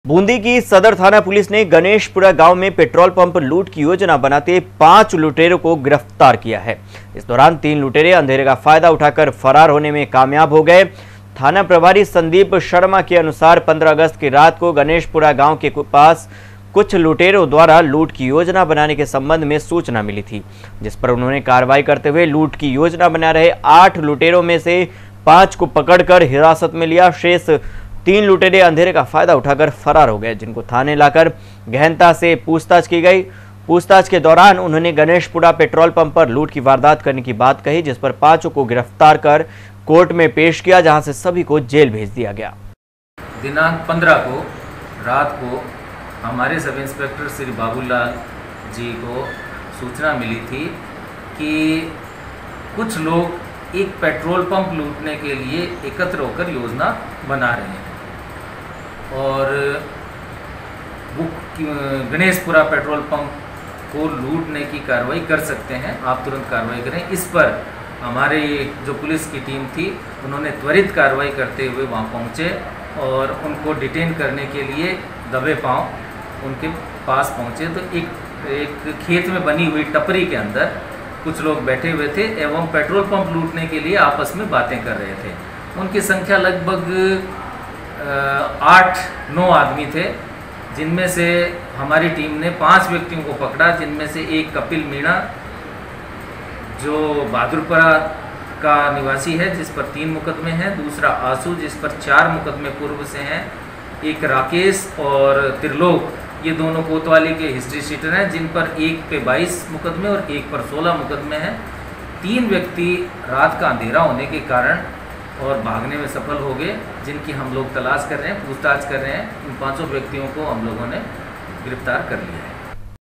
17 Aug. 21 @ news # पैट्रोल पंप लूट की योजना बनाते 5 जने धरे
पैट्रोल पंप लूट की योजना बनाते 5 जने धरे